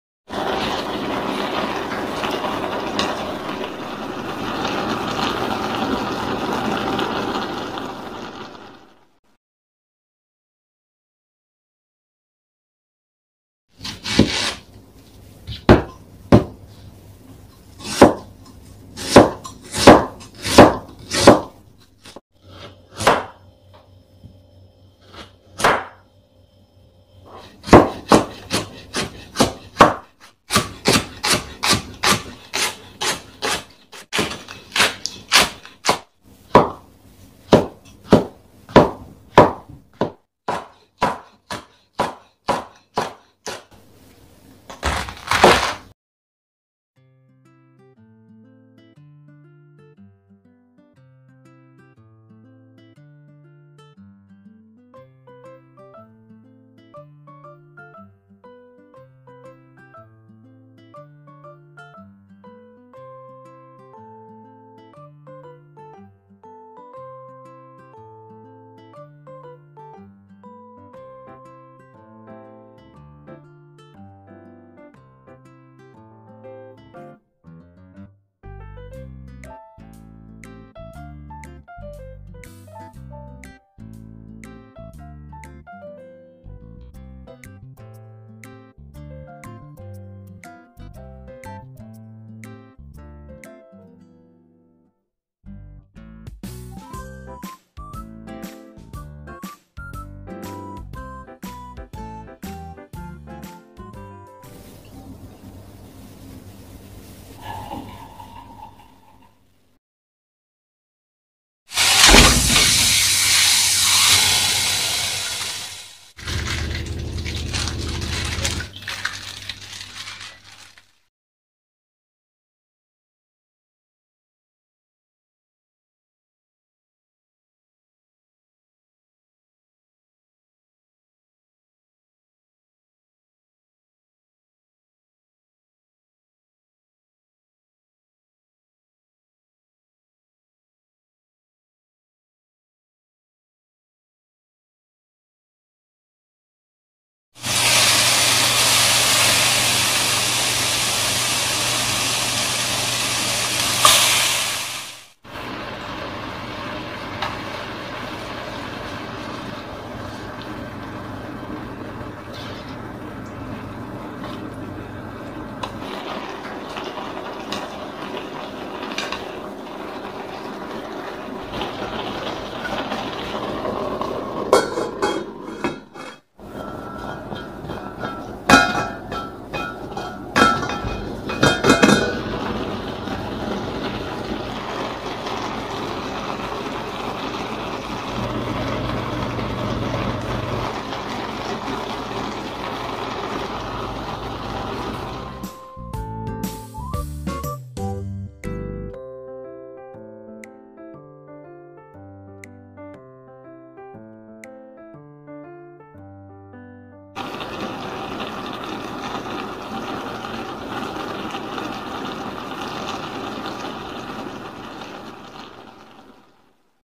[Bulgogi] 배를 갈아서 넣으면 실패하지 않아요 :: 소불고기를 맛있게 하는 방법
소불고기는 육질이 뻣뻣하면 맛이 없어요. 배를 갈아 넣으면 연육작용을 해서 고기가 연하고 배 특유의 시원한 단맛과 풍부한 과즙이 불고기의 맛을 좋게 해줍니다. 배를 갈아 맛있게 만들어 보세요~~!!

--- 필수재료---
양파 1개
대파 2대
팽이버섯 1개
불고기용 소고기 780g

--- 선택재료 ---
당근 50g
홍고추 1개
표고버섯 3개

--- 양념장 ---
배 1/2개
맛술 2tbsp (청주, 미림 사용 가능)
참기름 1tbsp
매실청 1tbsp (생략 가능)
올리고당 1tbsp (물엿, 조청 사용 가능)
다진마늘 2tbsp
후춧가루 약간
양조간장 6tbsp
자일로스 설탕 2tbsp(황설탕 등 사용 가능)

❤ Your support is a great help to produce more advanced videos